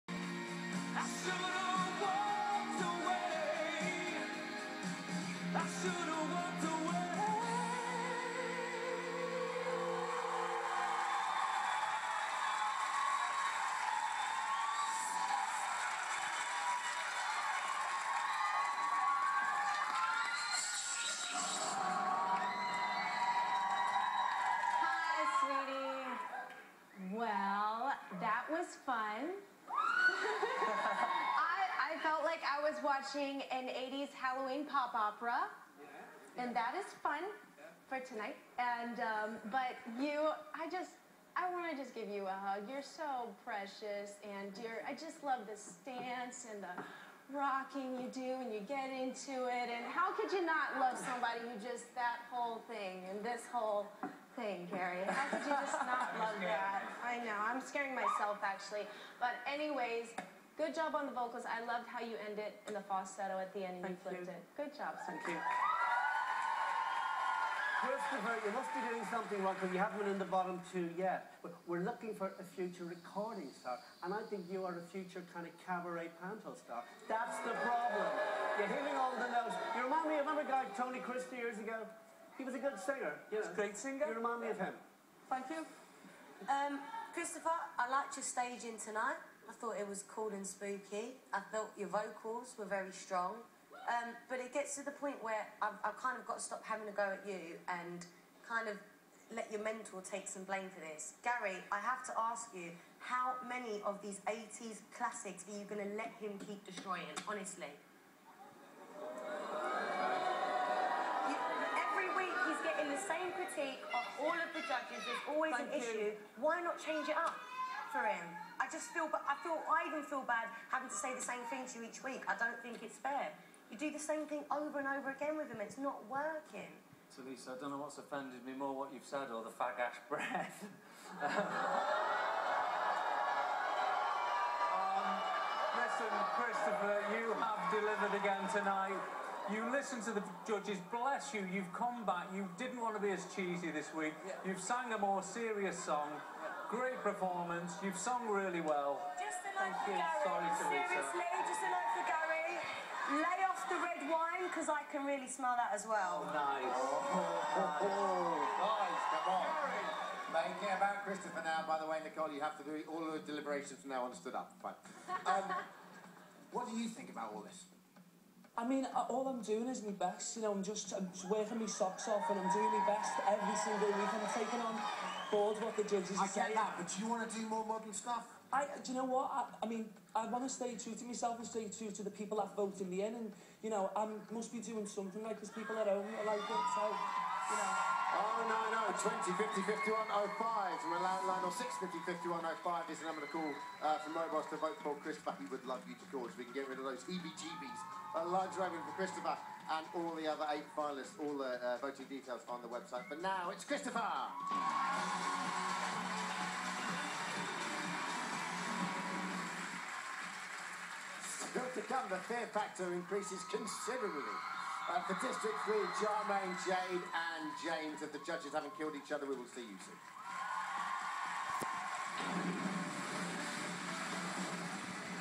Gary Barlow say's Tulisa has fag ash breath!
After Christopher Murpheys 80's performance yet again, Tulisa blamed mentor Gary Barlow. He wasn't sure what offended him most! The comment or the breath!